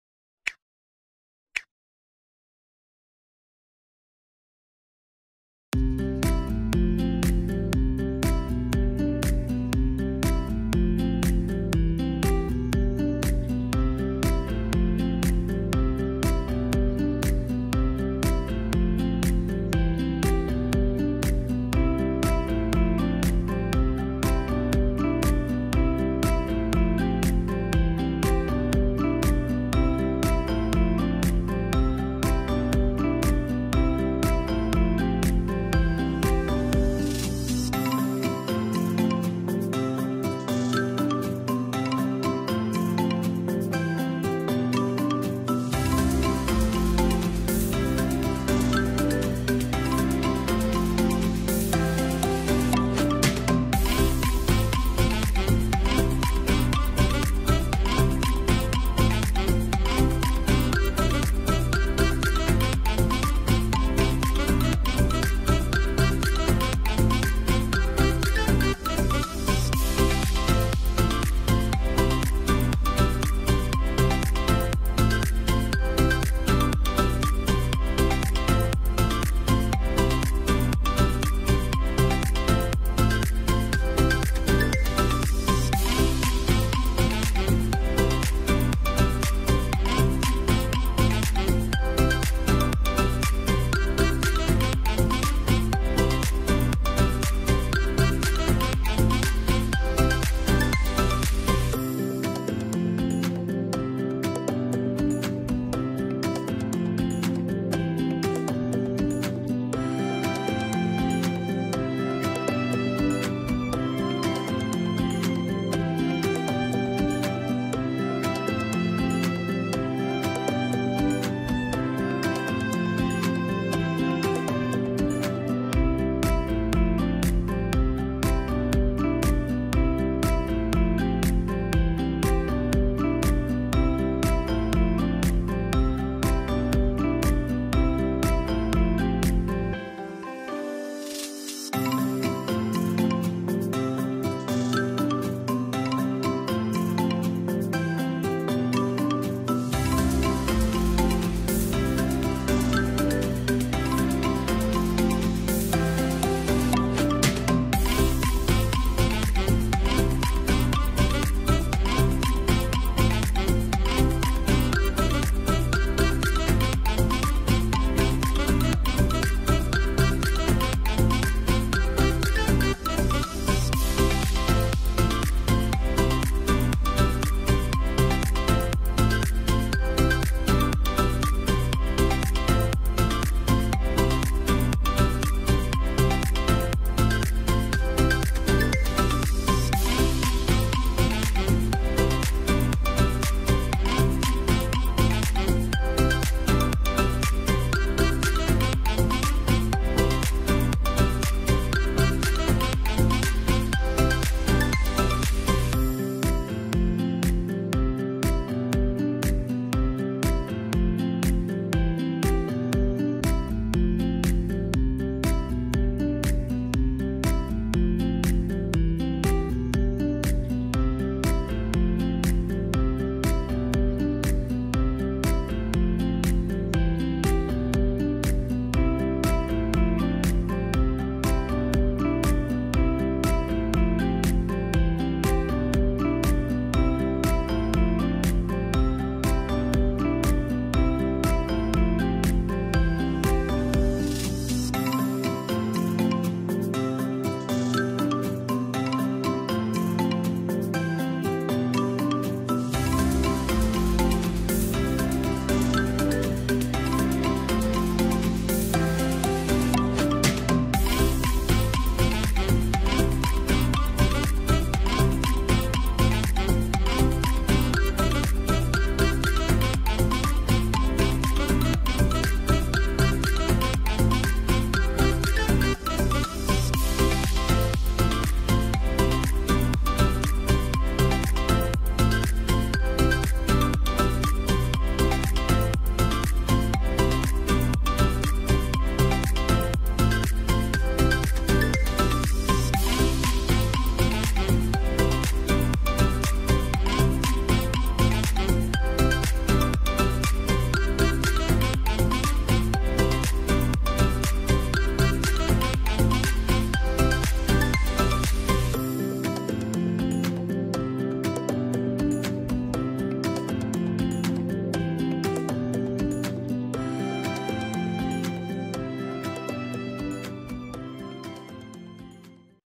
Line Cycle 8 Steps infographic Design: Illustrator Tutorial
Learn to design a  line cycle infographic using Adobe Illustrator by following this design tutorial